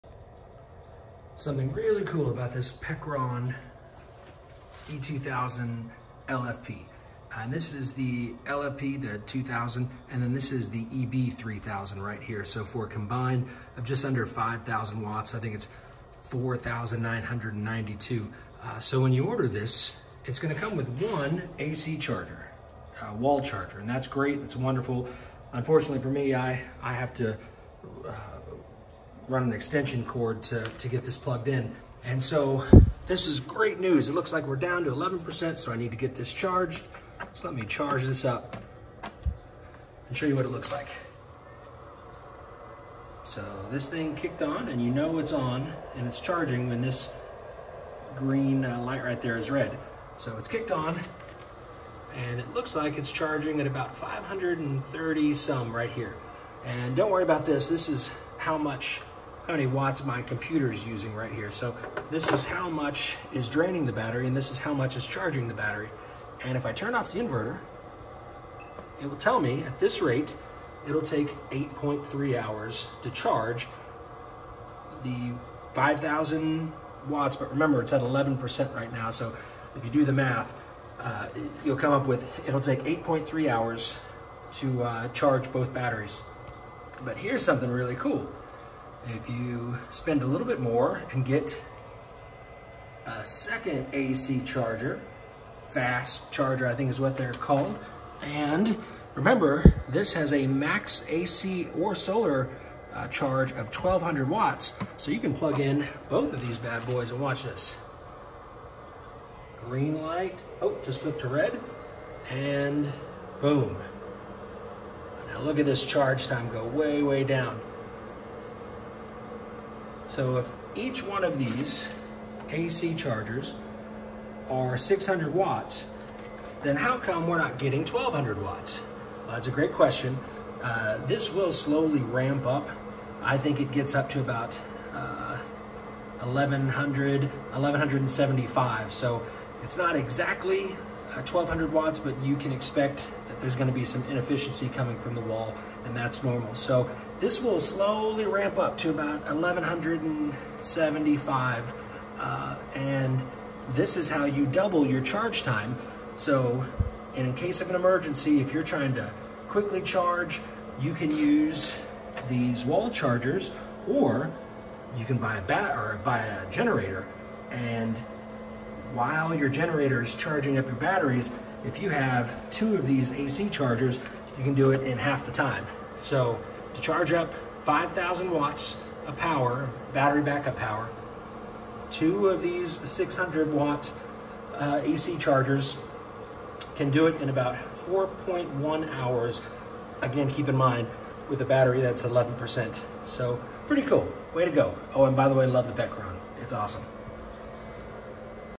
Fast Charging the Pecron E2000LFP & expandable EB3000 Power Station
This shows you why you should use two 600W AC Quick Chargers for the Pecron E2000 and EB3000 for fast charging.